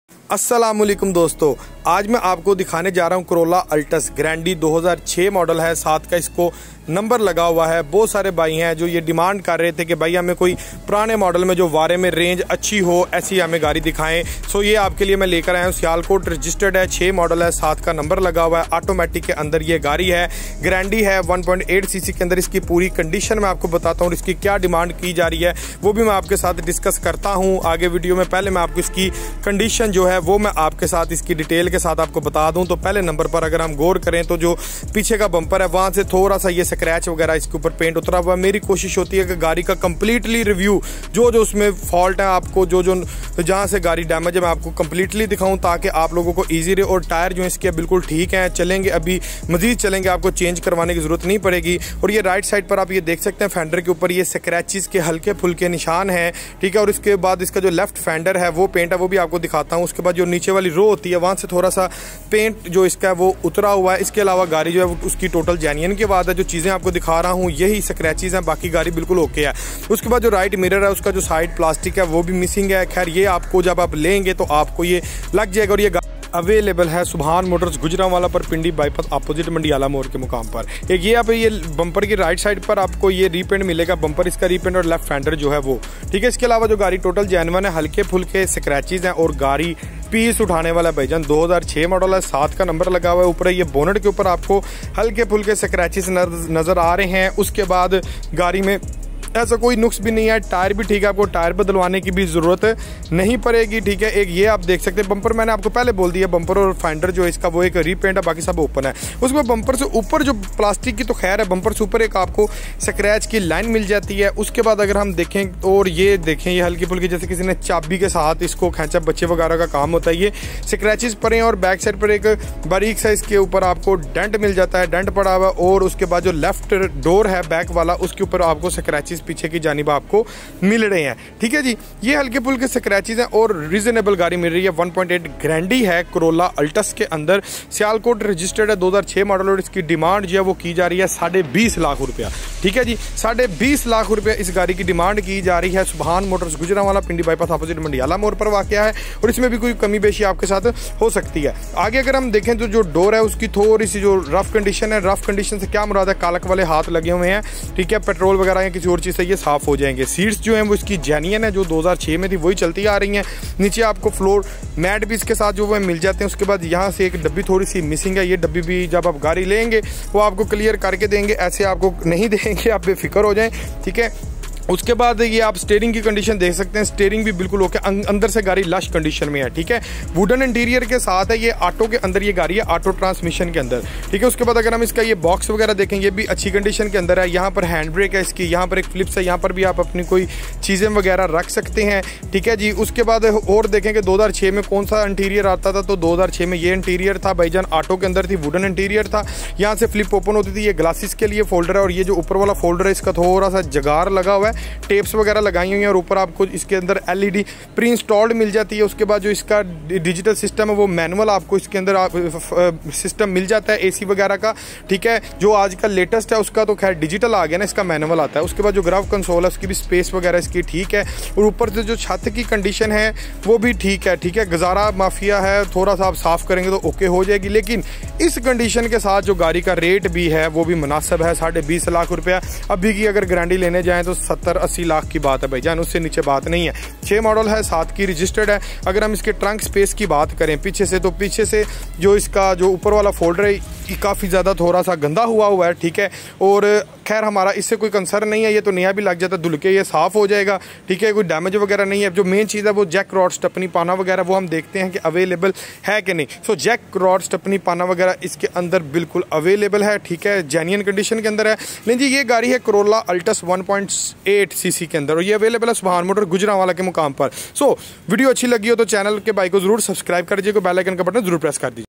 Toyota altis 1.8 cc 2006 model 2007 registered subhan motors Gujranwala used cars in pakistan
Toyota Corolla altis in  used condition 2006 model 2007 registered available on subhan motors Gujranwala at reasonable price located in pindi bypass OPPOSITE mandiala more gt road gujranwala